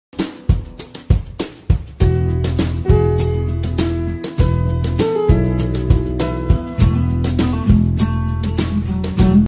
This Shorts Video Broke the Cargo Hold Chain of A Dry Dock Sprocket in the Merchant Navy
Sprocket opening of cargo hold chain link 🔗 at drydock in China zhoushan